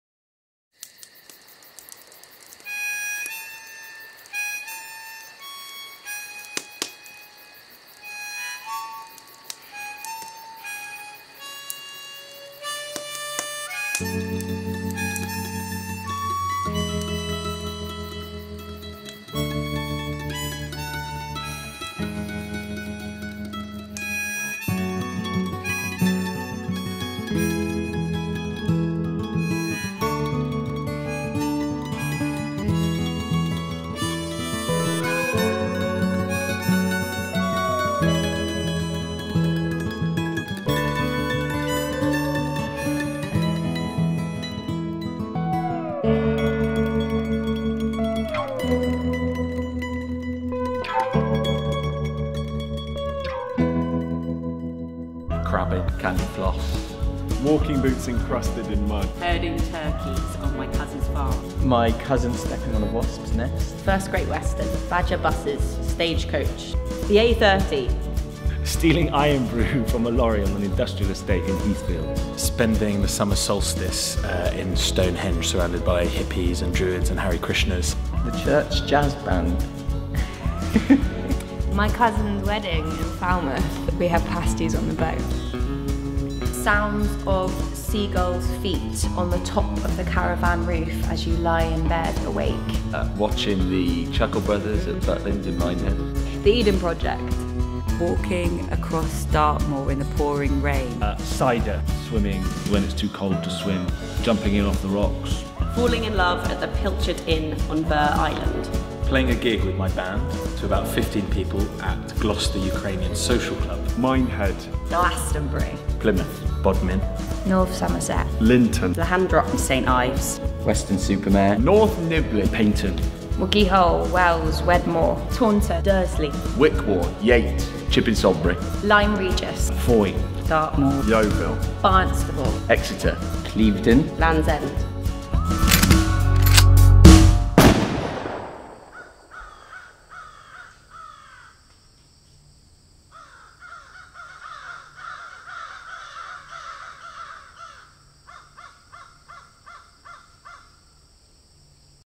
South Western Trailer
Award-winning Bristol company The Wardrobe Ensemble (Education,  Education, Education and 1972: The Future of Sex) present an odyssey of revenge from the mean streets of Bristol to the perilous cliffs of Cornwall. Runs at Tobacco Factory Theatres Tue 17 - Fri 27 July 2018.